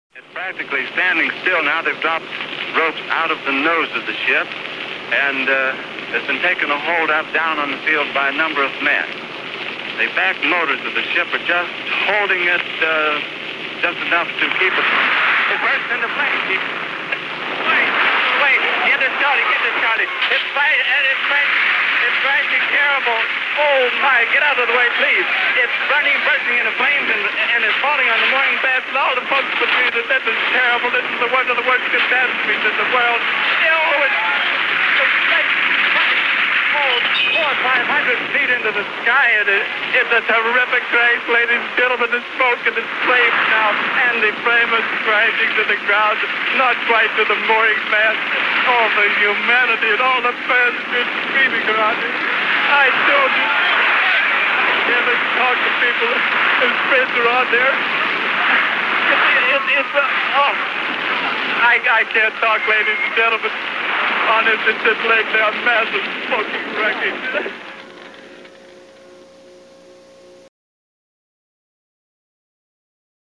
Hindenburg Disaster, Hydrogen Airship Explodes.mp4
The Hydrogen Airship Explodes, known as the Hindenburg Disaster.